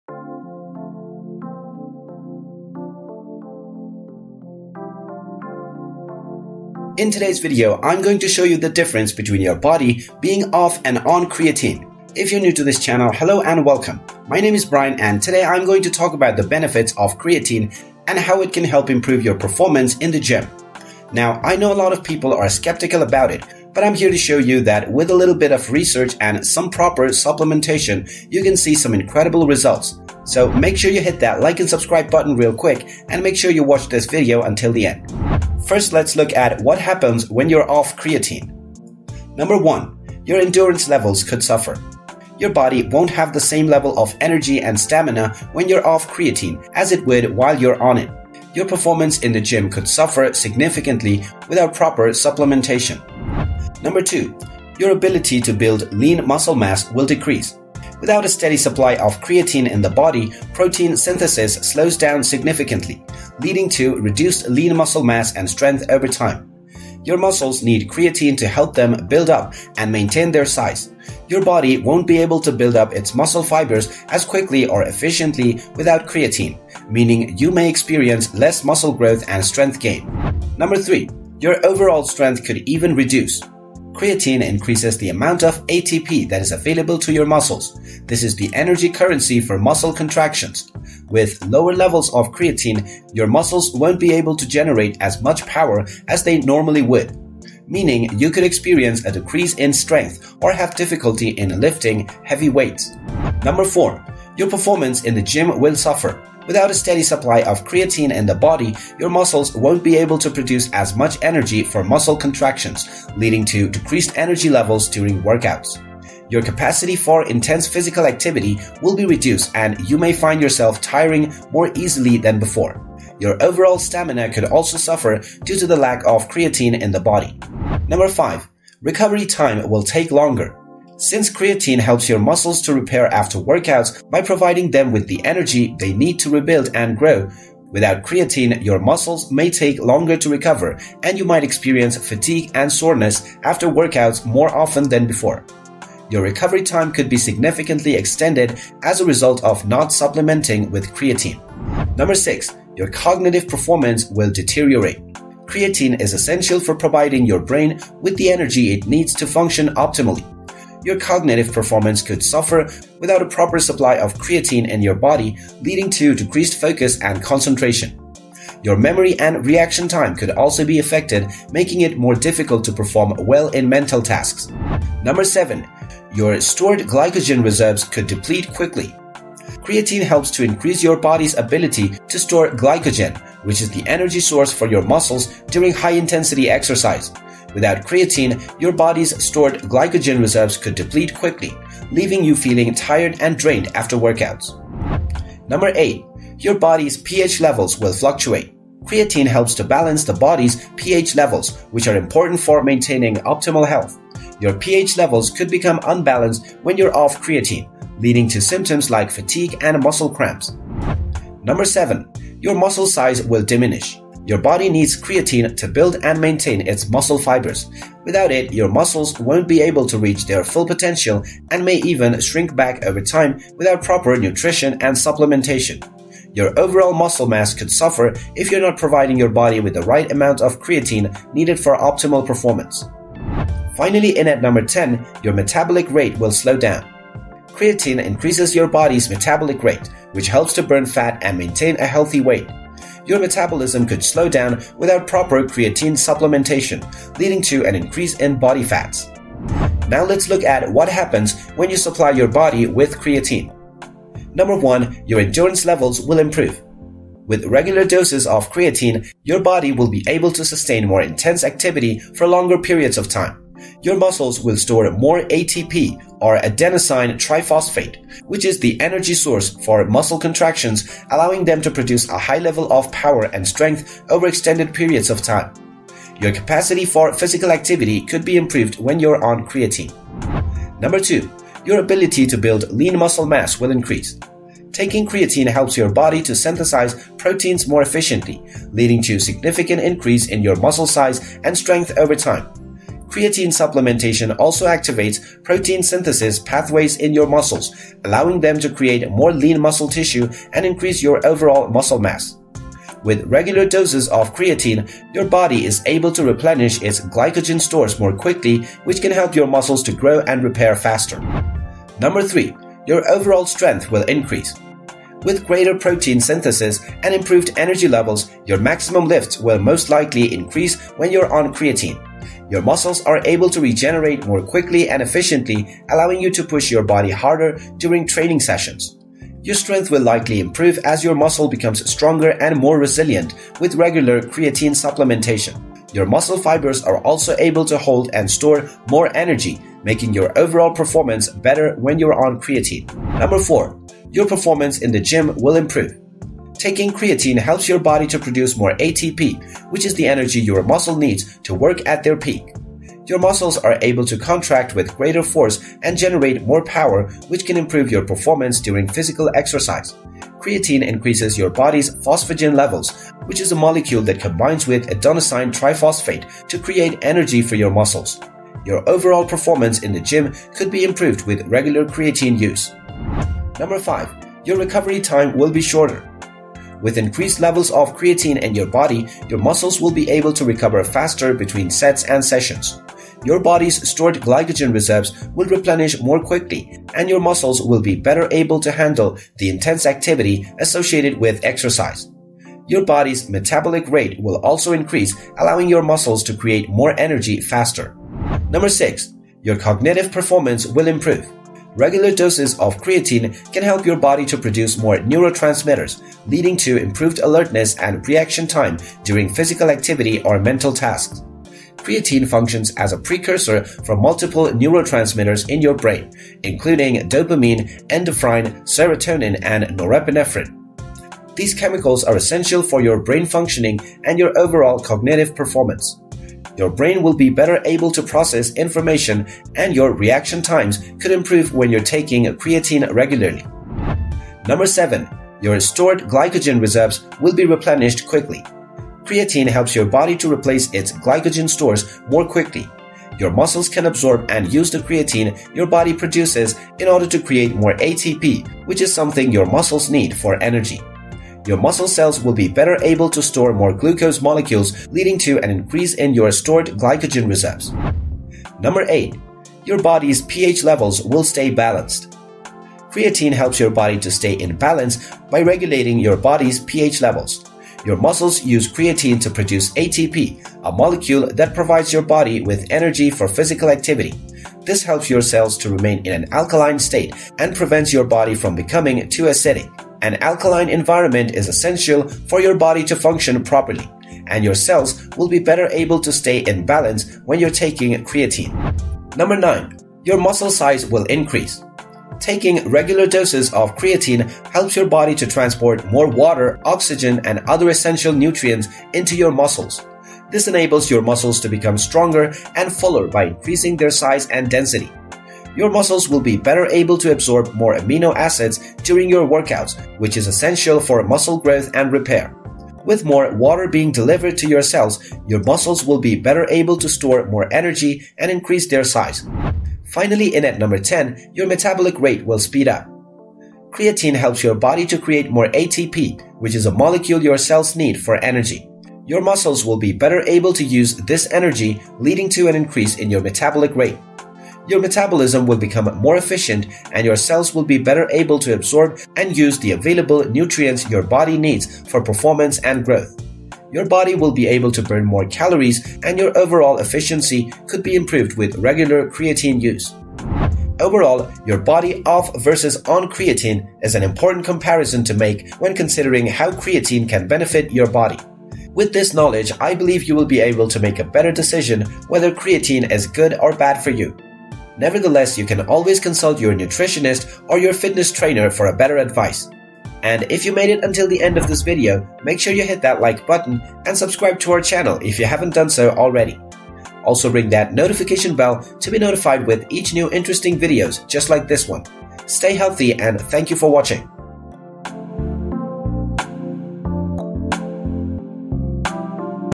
Creatine vs No Creatine Body: Effects of Creatine on Our Body
Do want to use creatine to boost your workout limits and also to boost your muscle growth.
We will be watching the effects of creatine on the body both with creatine vs no creatine body.
The differences between a body with creatine vs no creatine can be amazing. And also, how taking creatine can impact your physical performance and overall health. We'll look at the science behind creatine, including how it works in the body and how it can be beneficial and non beneficial for athletes, bodybuilders, and anyone looking to improve their fitness level.

Do you want more Videos like this?